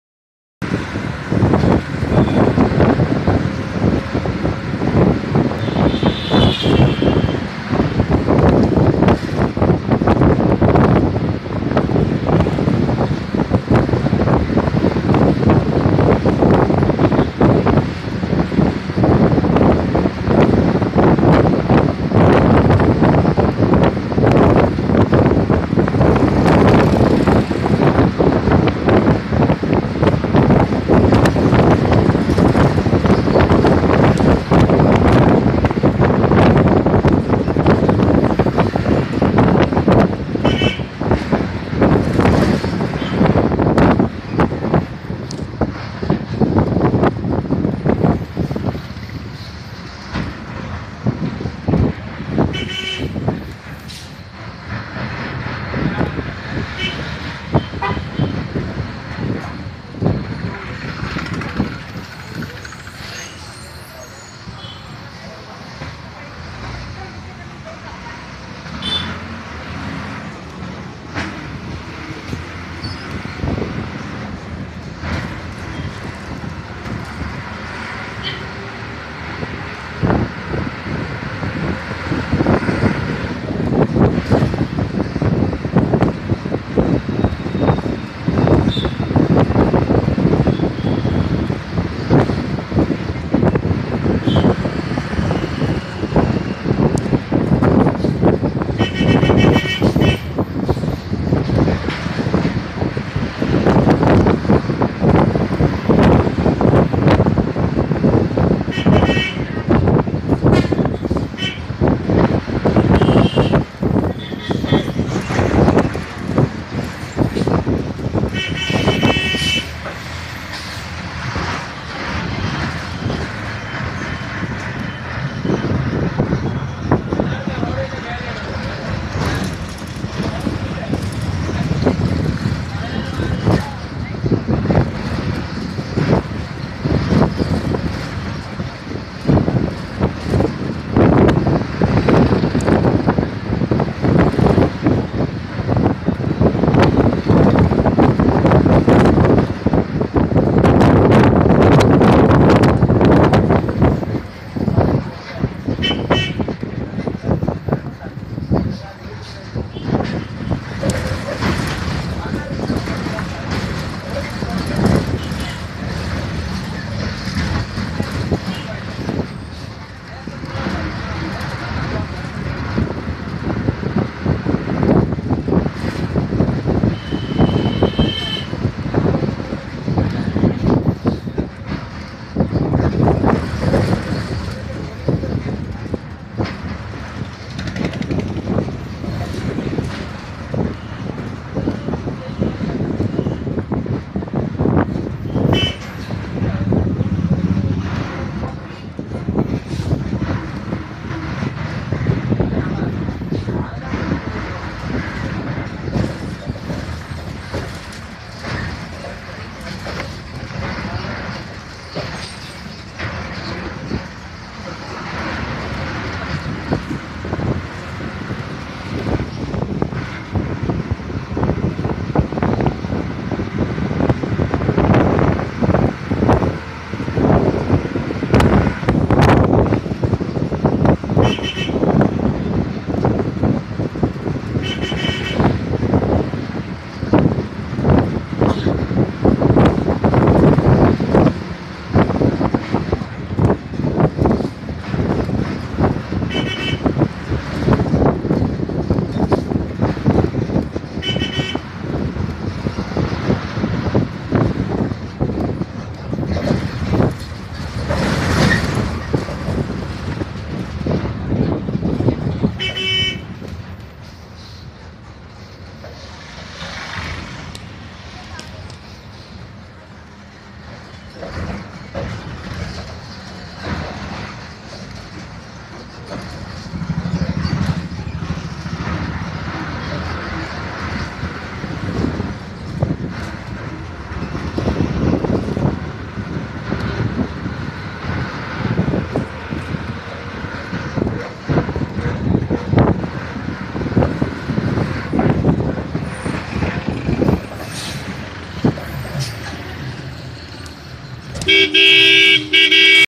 RSM999 PPD KSRTC bus journey part 5.
Trivandrum - Nagercoil FP. Via Balaramapuram . Neyyattinkara . Parassaala . Marthandam . Azhagiyamandapam . Thuckala . Villukuri . Parvathipuram . Scot chiristian college . Collectrate . WCC College .